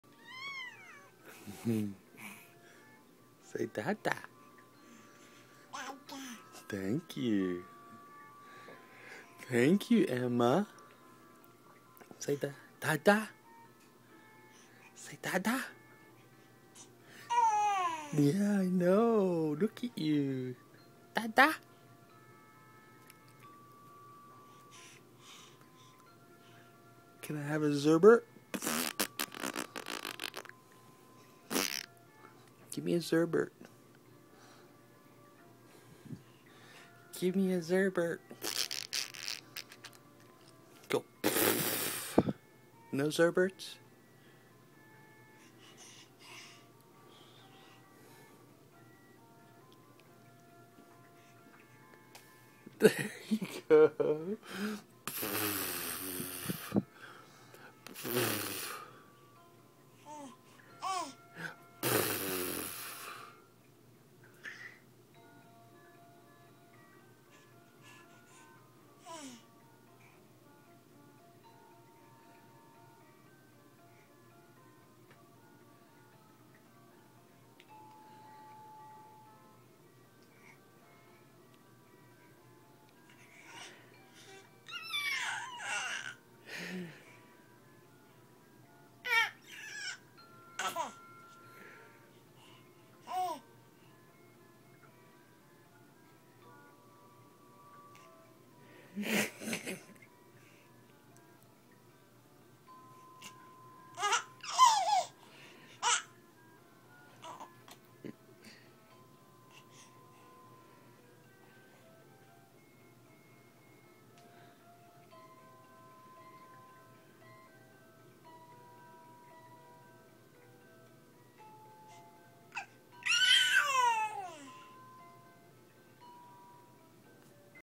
Emma and her toys
She's being babysat by the Big Bear